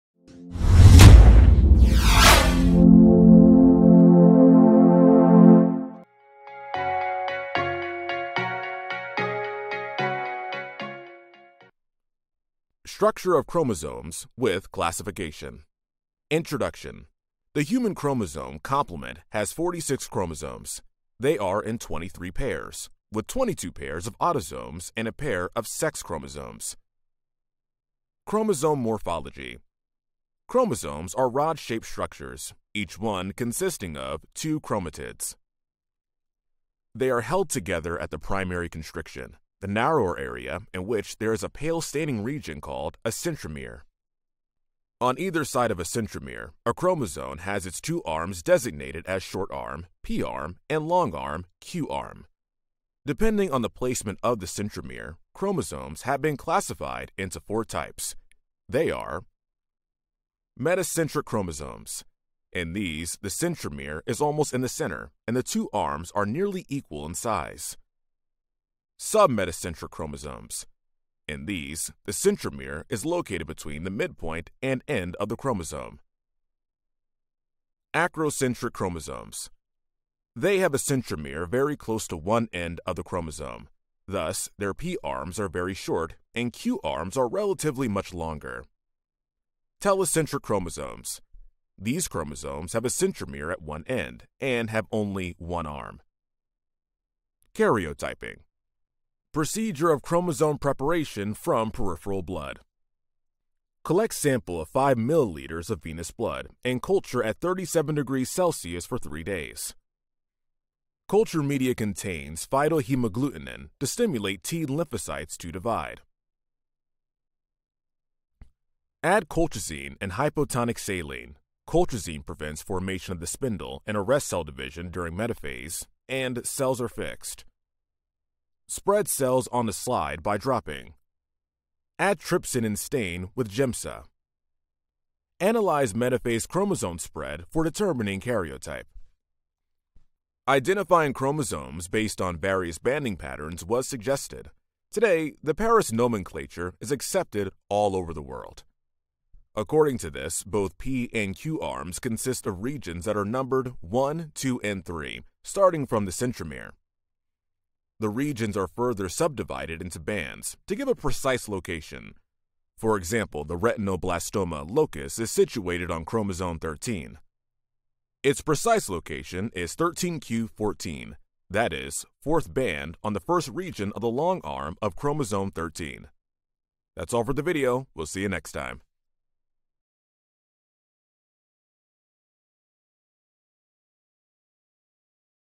Structure of Chromosomes: Chromosome morphology & Karyotyping : USMLE Step 1
Structure of Chromosomes: Chromosome morphology & Karyotyping - AN 73.1 MBBS CBME Curriculum

Chromosomes are long, thin, and thread-like structures that carry genetic information in the form of DNA. They are located in the nucleus of a cell and are visible under a microscope during cell division.

The structure of a chromosome can be divided into several components. The two main components are the chromatin and the centromere. Chromatin is a complex of DNA and proteins that form the bulk of the chromosome. The centromere is a region of the chromosome that holds the two sister chromatids together.

There are different types of chromosomes based on their morphology or appearance. The most common types of chromosomes are metacentric, submetacentric, acrocentric, and telocentric.

Metacentric chromosomes have a centrally located centromere and two arms of equal length.
Submetacentric chromosomes have a centromere that is slightly off-center, giving the chromosome two arms of different lengths.
Acrocentric chromosomes have a centromere that is located near one end of the chromosome, resulting in one very long arm and one very short arm.
Telocentric chromosomes have a centromere located at the very end of the chromosome, resulting in a single long arm.
Karyotyping is a laboratory technique used to identify and analyze the number and structure of chromosomes in an individual's cells. During karyotyping, cells are treated with a chemical that stops them from dividing at the metaphase stage of cell division, when the chromosomes are most condensed and visible. The cells are then stained and examined under a microscope, and the chromosomes are arranged in order according to their size, shape, and banding pattern. This allows for the detection of chromosomal abnormalities, such as missing or extra chromosomes, or structural rearrangements, which can be associated with genetic disorders.